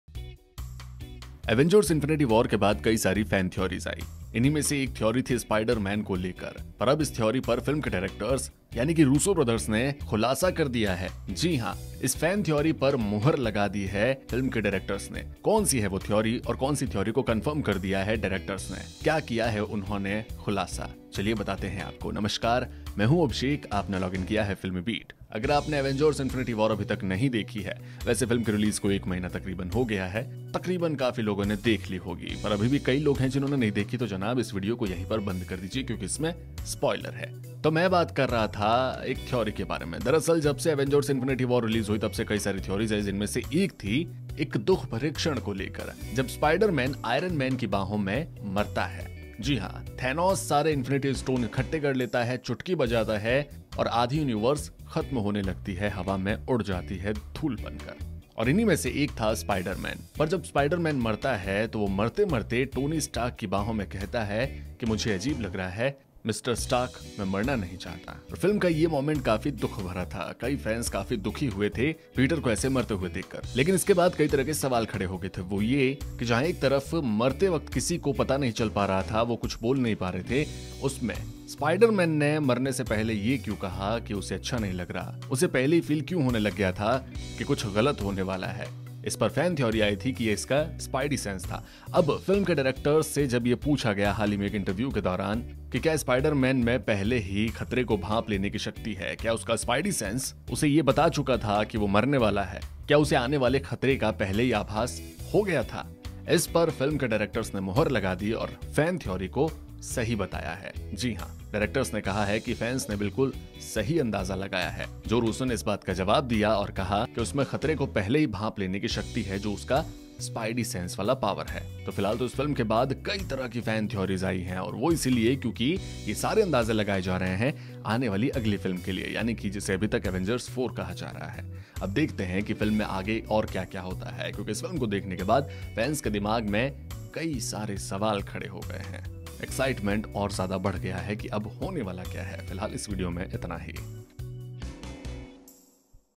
Avengers Infinity War: Director's Shocking REVELATION on Spider Man | FilmiBeat
Avengers: Infinity War has been creating history with its collection but the movie also probably left you with some big unresolved questions. Like how Spider Man survived till and and have some dialogues ? But now film's Directors Joe and Anthony Russo revealed this mystery.